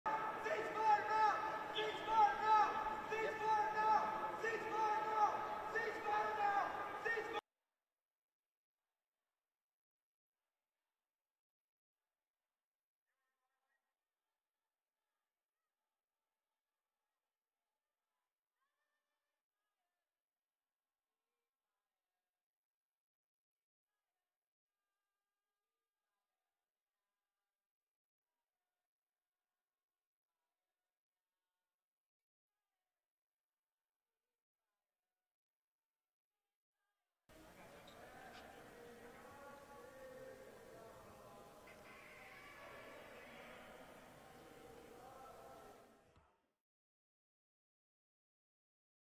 Demonstration at US Senate Office Leads to Arrests | VOA News
Capitol Police arrested more than 40 protesters demonstrating at a Senate office building Monday.

The demonstrators were calling for a cease-fire in the war between Israel and Hamas.

One protester climbed the Mountains and Clouds Sculpture in the atrium of Hart Senate Office Building and tied himself to the sculpture.

Around 270 Palestinians have been killed in the West Bank since the October 7 Hamas attack on Israel and the launch of the Israeli offensive to eradicate Hamas in the Gaza Strip.

The World Health Organization called Tuesday for the “protection of health care and humanitarian assistance in Gaza.” It cited difficulties a WHO team encountered while trying to deliver medical supplies to a hospital in Gaza City and to evacuate critically hurt patients, saying there were delays at military checkpoints and that workers from the Palestinian Red Crescent Society were detained.